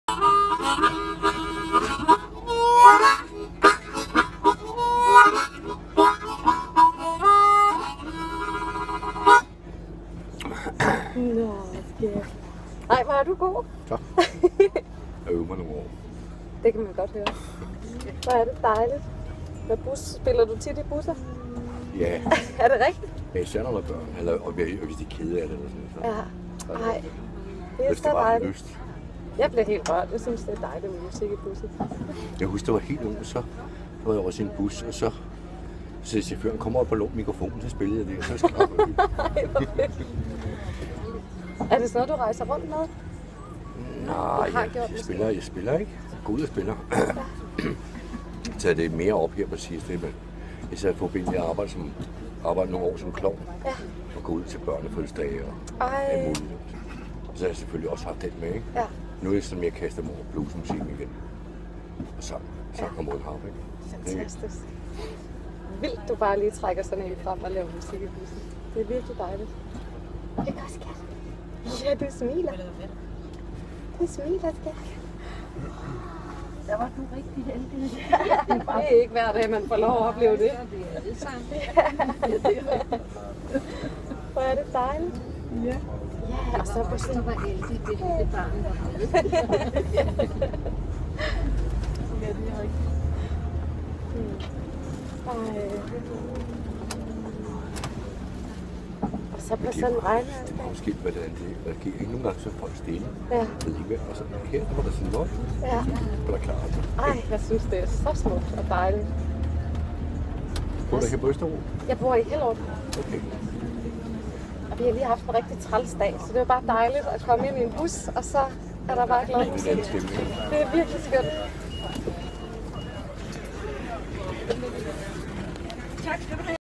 Go' buskarma og mundharmonika i bussen 1A Østerbro
Jeg stod i en propfyldt bus med en lidt ked af det datter. Ved siden af mig stod en ældre herre, og han begyndte at snakke med min datter. Jeg spurgte om han ville holde hende, han virkede så rar. Da jeg fik hende tilbage trak han en mundharmonika op ad lommen og begyndte at spille blues, han var fantastisk! Hele bussen klappede og jeg begyndte at græde fordi jeg blev så rørt. Hverdagshelten ❤️